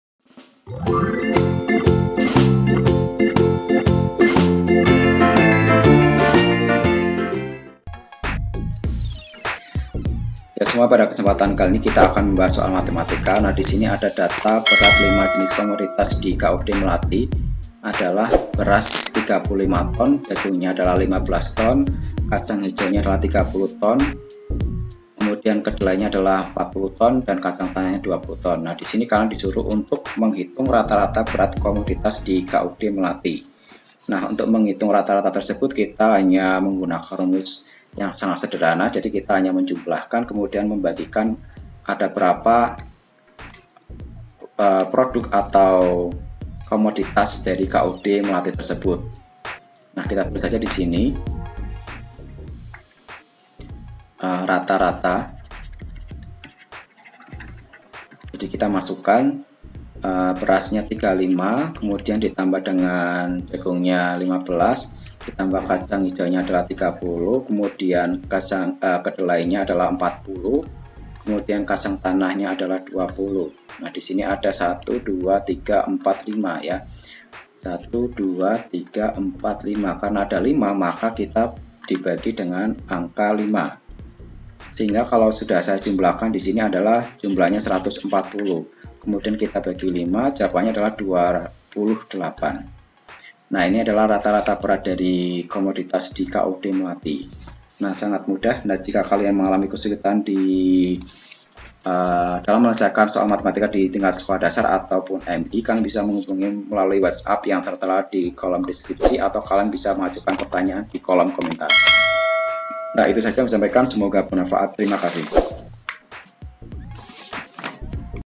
Data berat lima jenis komoditas di KUD Melati adalah beras 35 ton
JUDUL :
Data berat lima jenis komoditas di KUD Melati adalah beras 35 ton, jagung 15 ton, kacang hijau 30 ton kedelai 40 ton, dan kacang tanah 20 ton. Rata-rata berat komoditas di KUD Melati​  adalah... ton